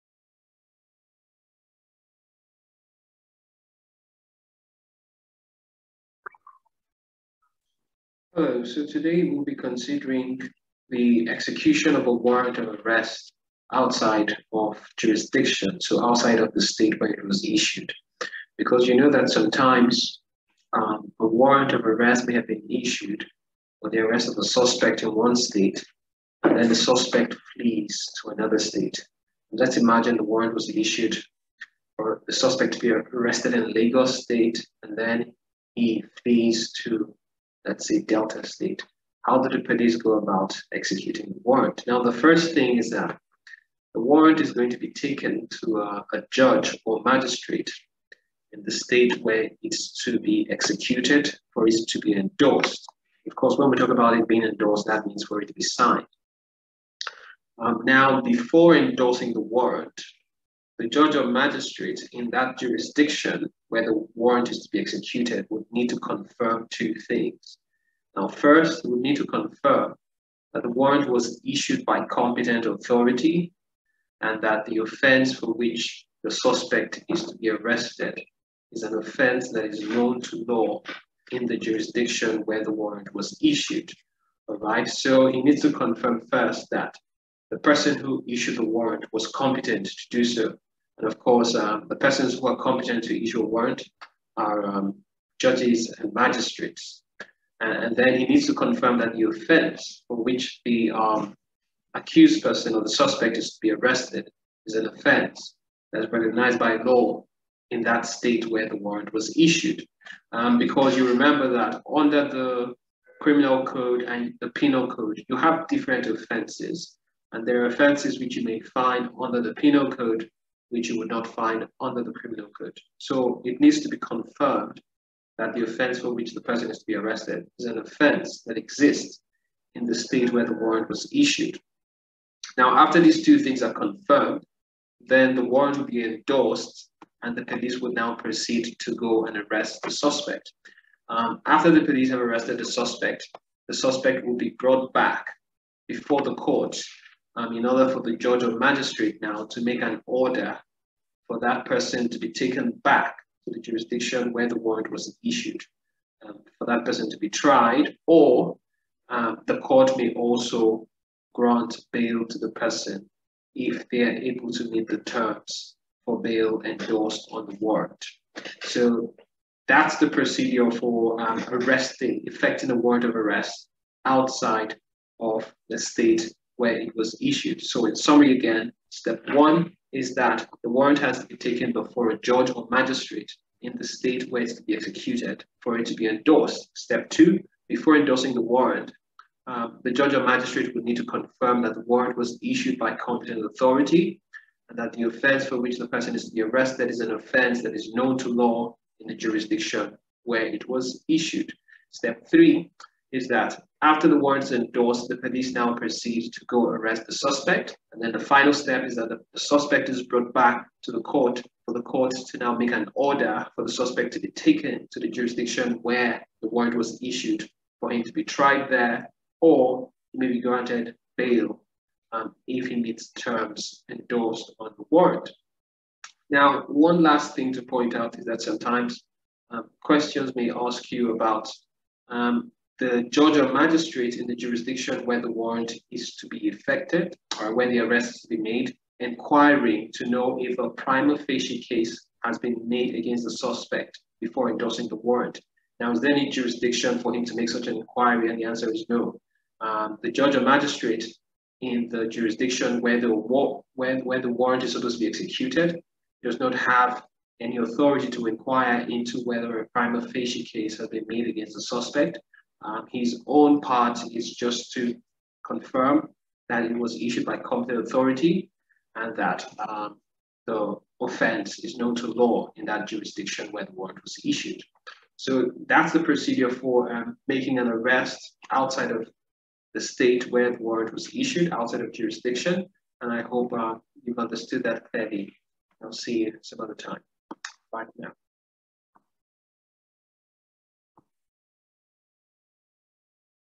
Executing a warrant of arrest outside of Jurisdiction
Criminal Litigation, Nigerian Law School Bar 2
Learn about the procedure for executing a warrant of arrest outside the state of issuance